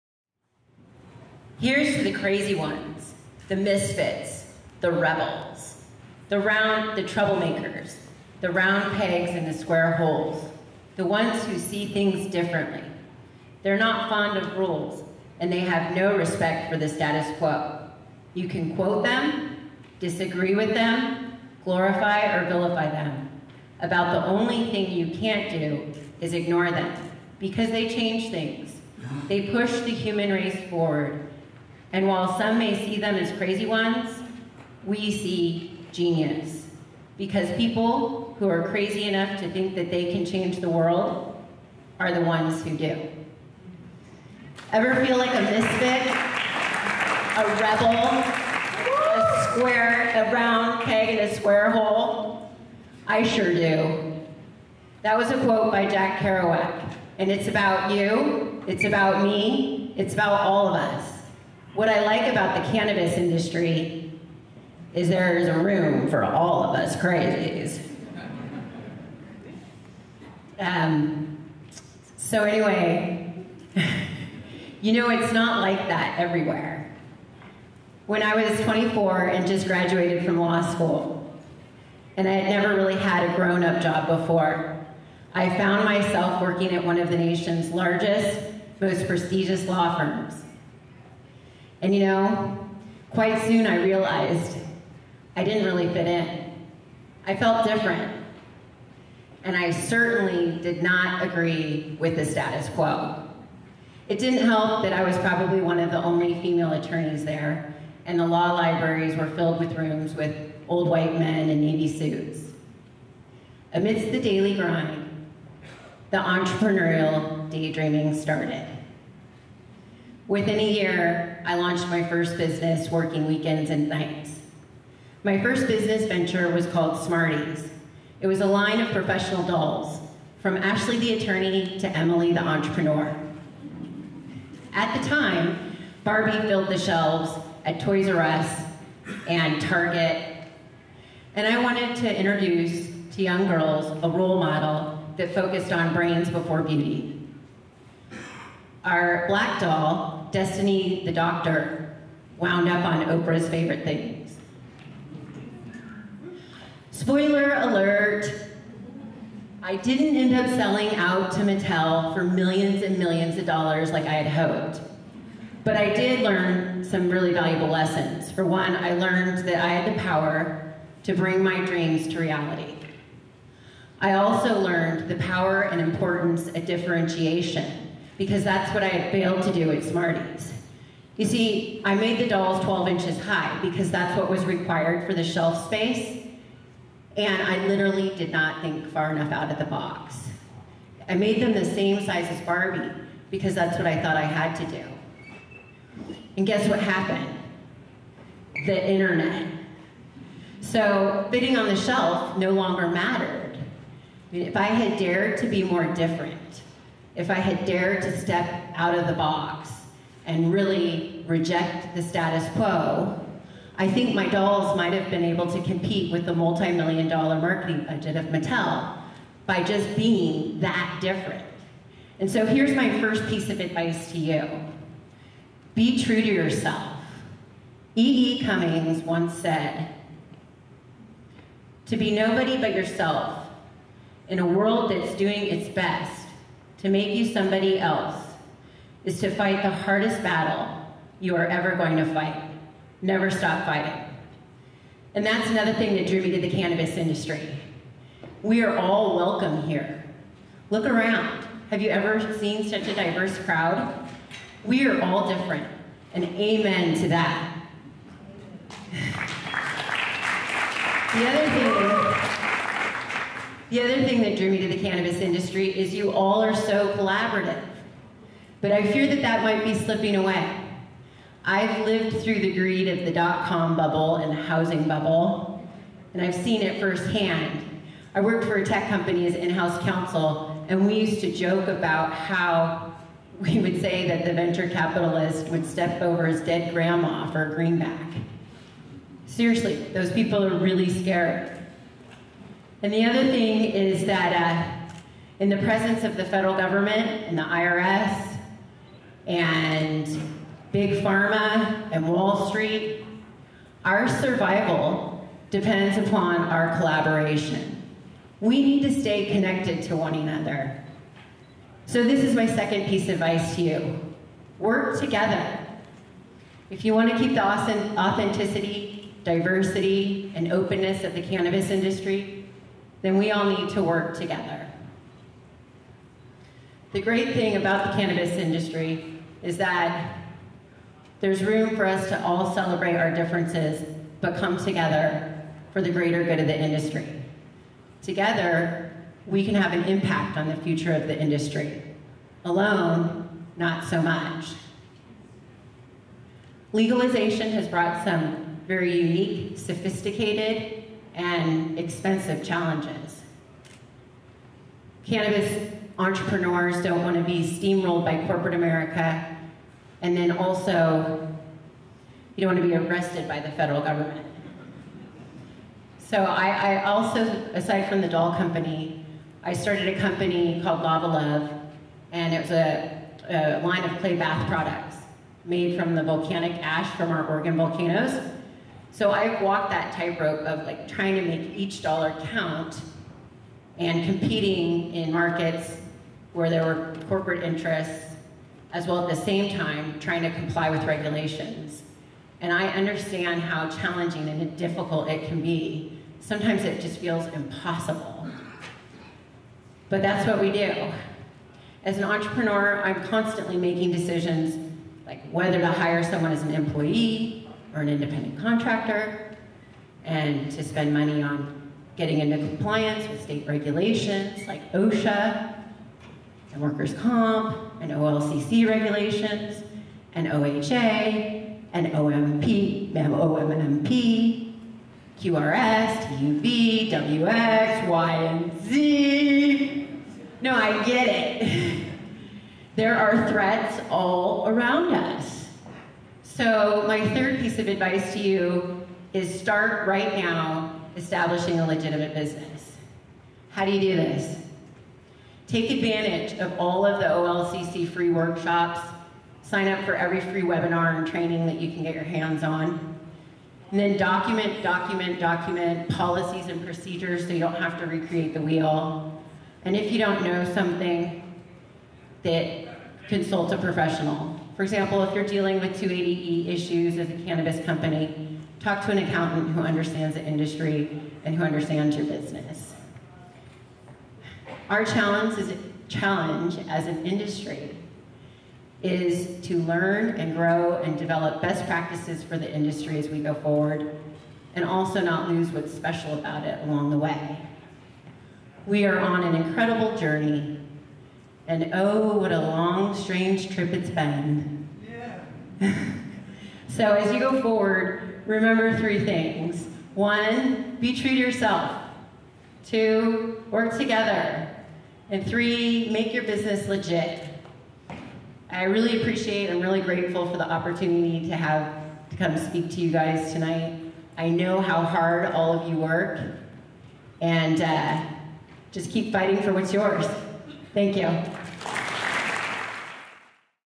OCA Toke Talks: Jennifer Clifton of Clifton Cannabis Law
The Oregon Cannabis Association and the Portland Mercury present Toke Talks at the Aladdin Theater on April 24, 2017.